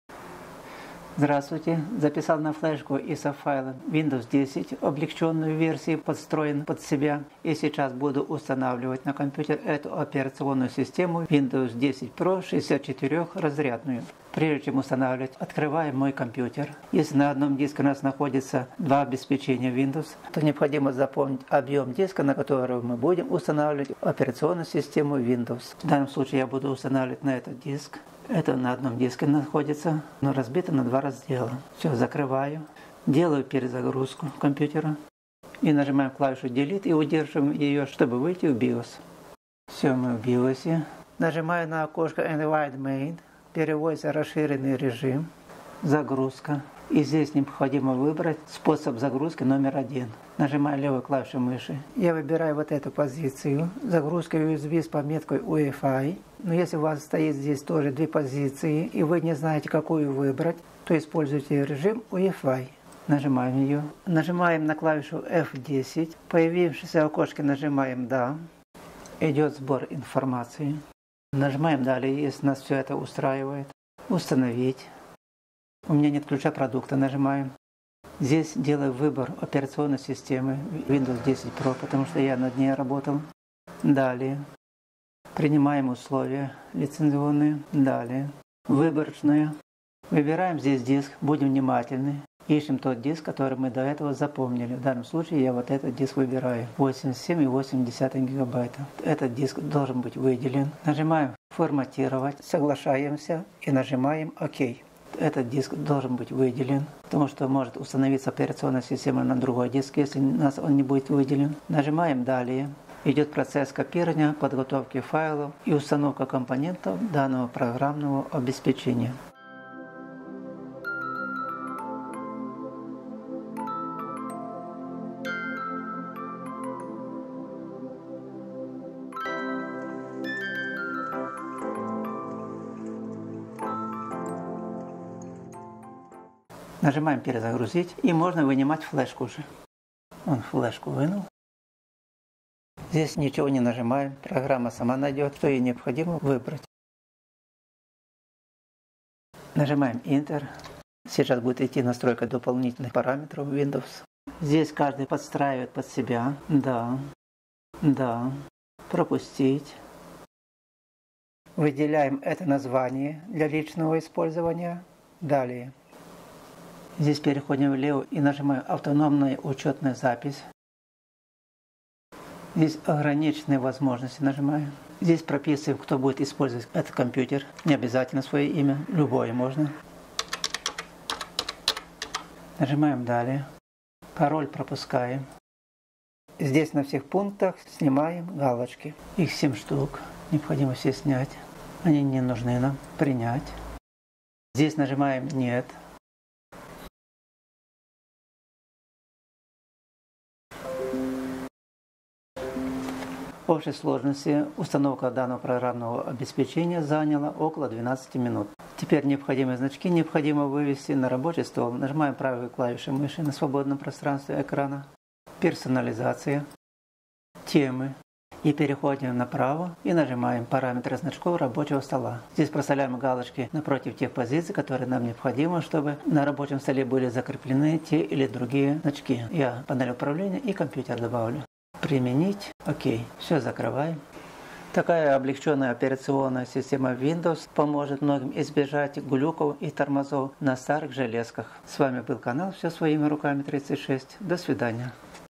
Как установить виндовс 10 с флешки через биос.Какие ошибки нельзя допускать при установке виндовс
Как установить виндовс 10 с флешки через биос правильно,что бы избежать ошибки при установке её.Какие ошибки нельзя допускать при установке виндовс на компьютер или ноутбук.